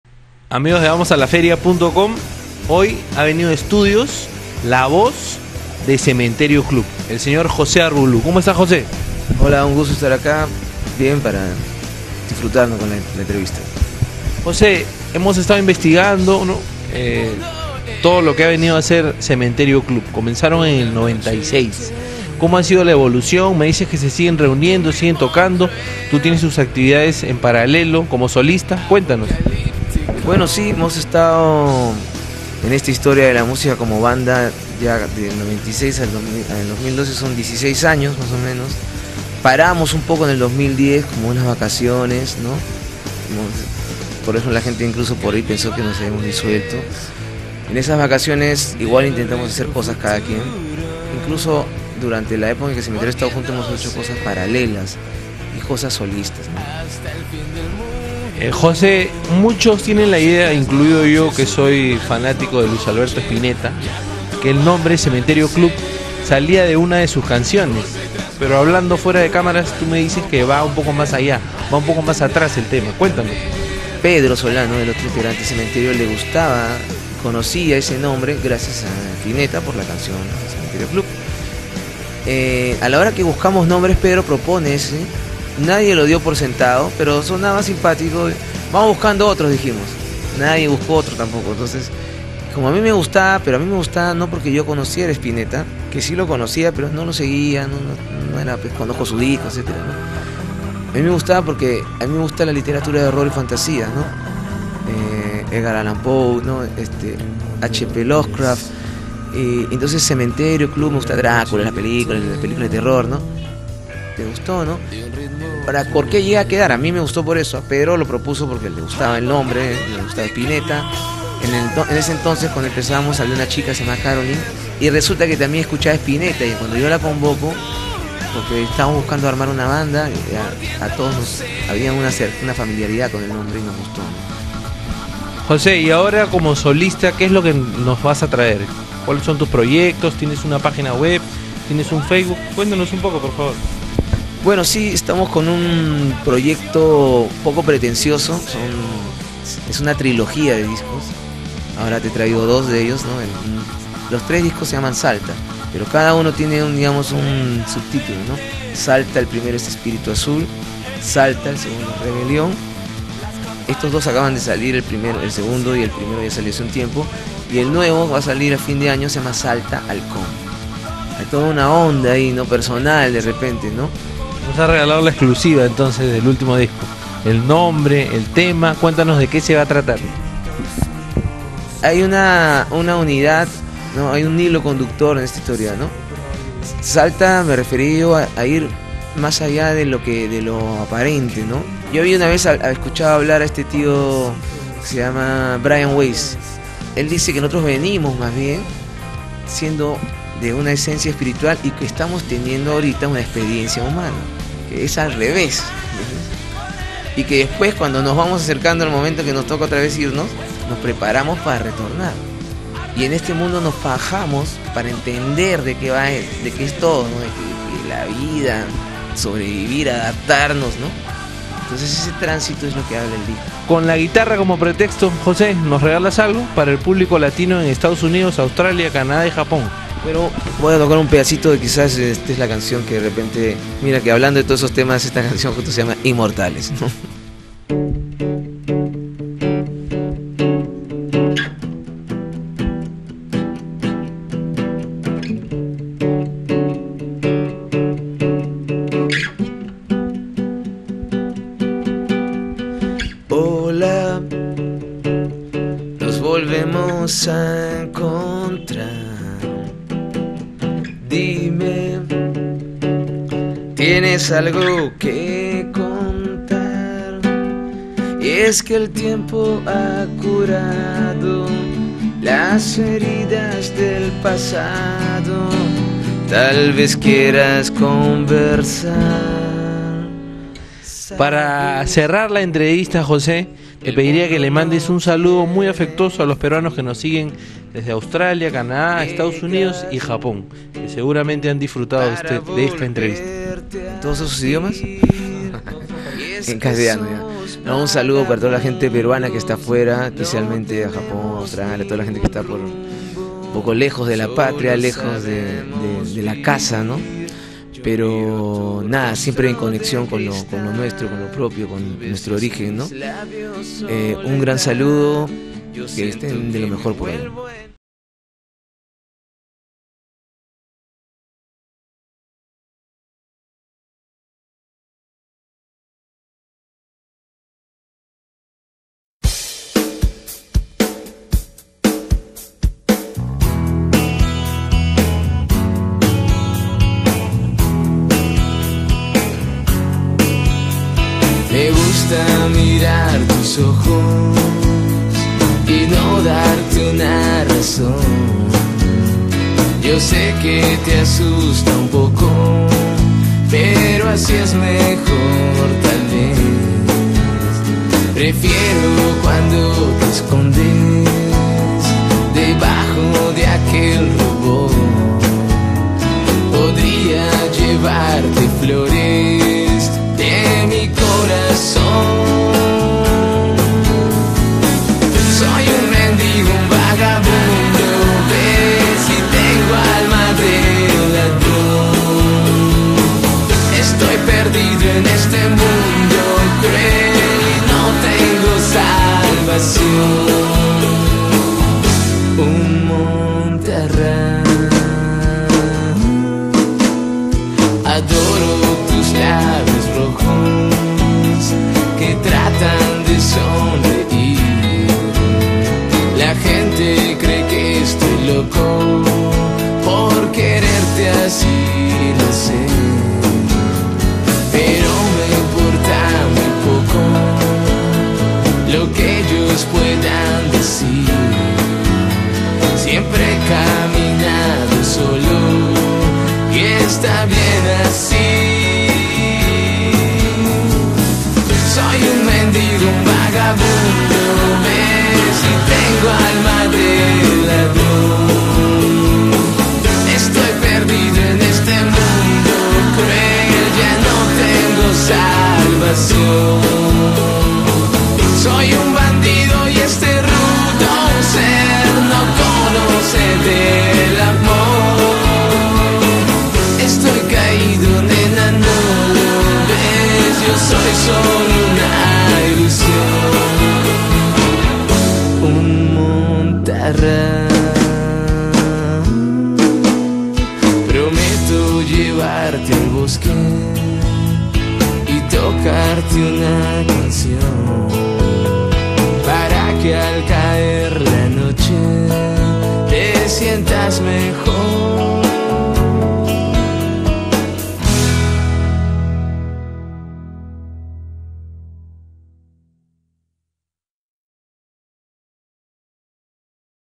Cementerio Club - José Arbulú
Cantante nacional nos cuenta la historia de la banda y también nos cuenta sus planes como solista.